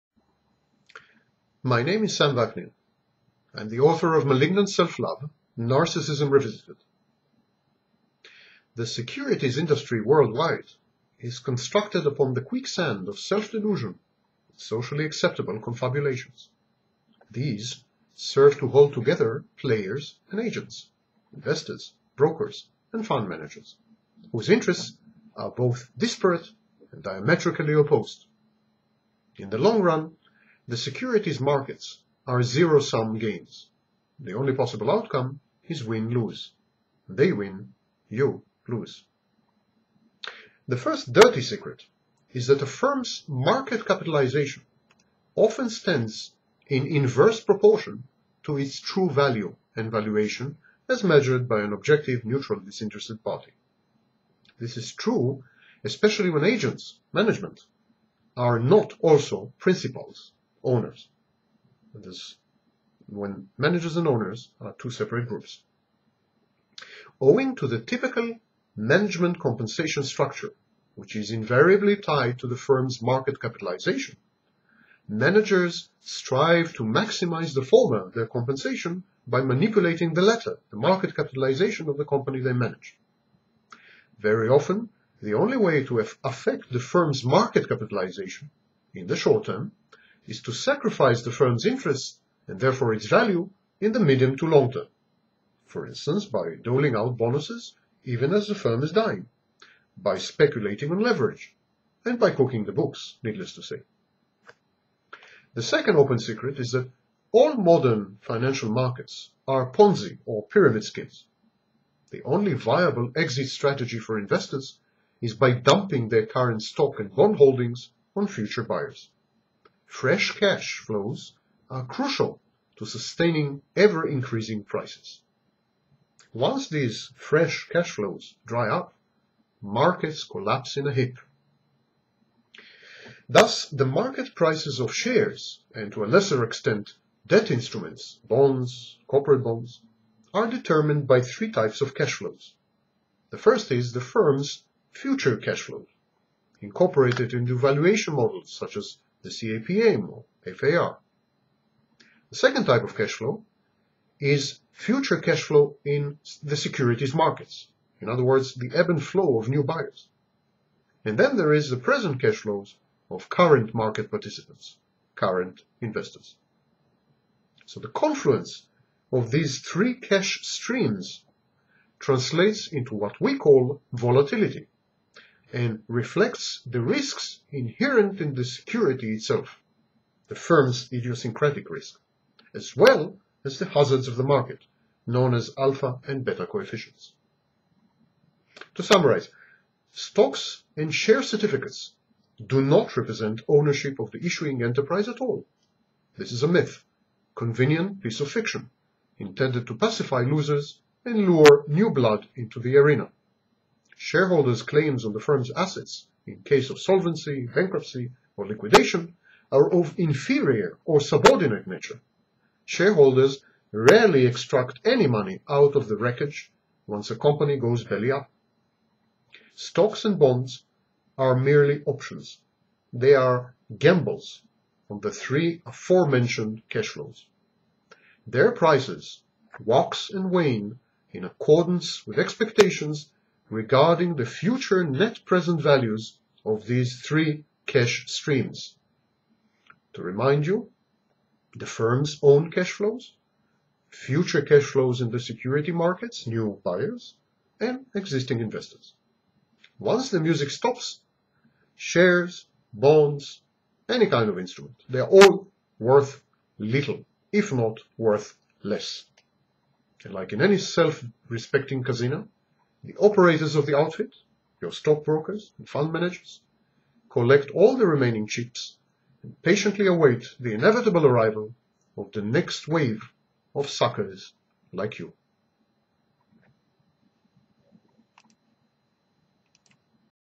Things Your Stockbroker Will Never Tell You
The securities industry worldwide is constructed upon the quicksand of self-delusion and socially-acceptable confabulations. These serve to hold together players and agents (investors, brokers, and fund managers) whose interests are both disparate and diametrically opposed. In the long run, the securities markets are zero-sum games and the only possible outcome is win-lose: they win, you lose.